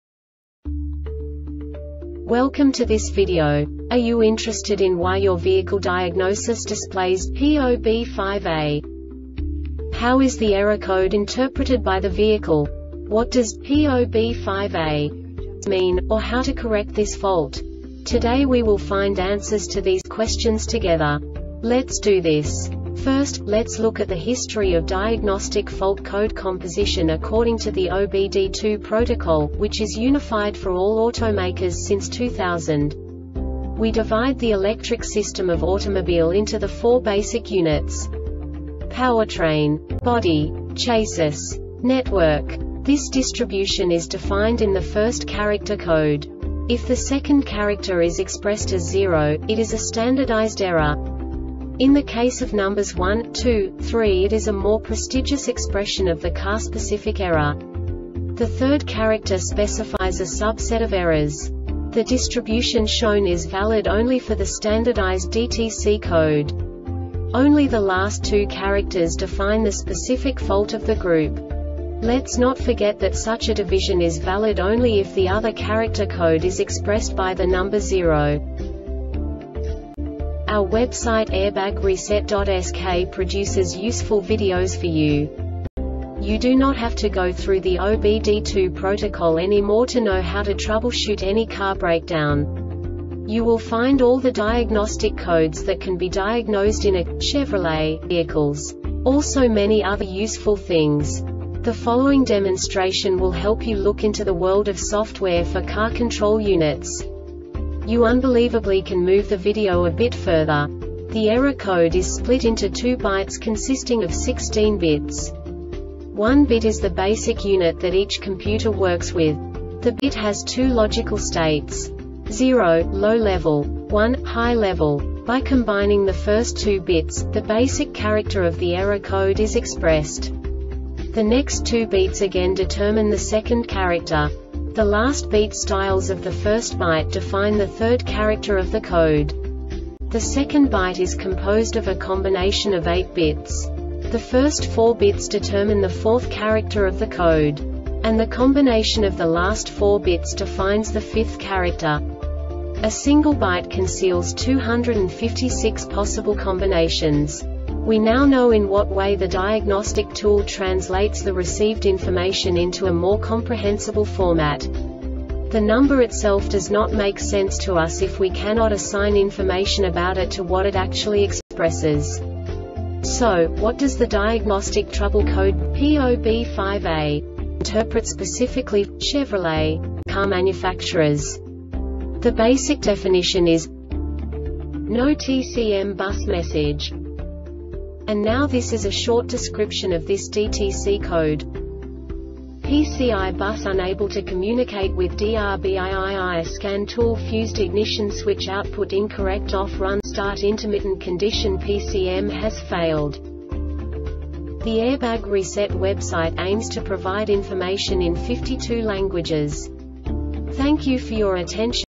DTC Chevrolet P0B5A Short Explanation
Contents:
0:21 Basic DTC analysis according to OBD2 protocol standard.
1:48 Insight into programming
2:58 A diagnostically understandable description of the Diagnostic Trouble Code
3:05 Basic error definition

Make:
Chevrolet
Code:
P0B5A
Definition:
No TCM Bus Message
Cause: